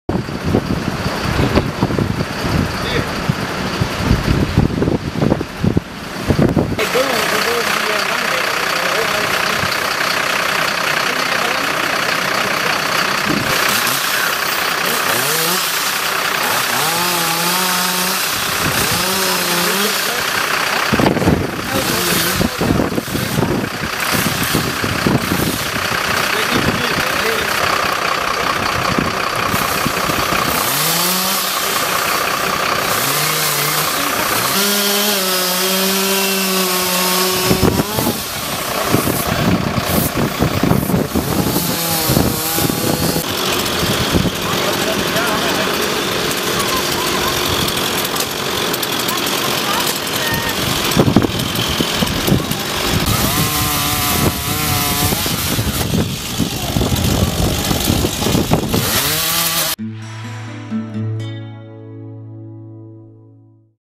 Brandweer is in hele provincie druk met gevolgen van harde wind
De brandweer in Enschede had vanmiddag de handen vol aan de gevolgen van de harde wind. Aan de Klaaskateweg raakte een dak van een woonhuis beschadigd en in het Wooldrikspark dreigde een boom om te vallen. In Giethoorn blokkeerde een omgewaaide boom de Beulakerweg.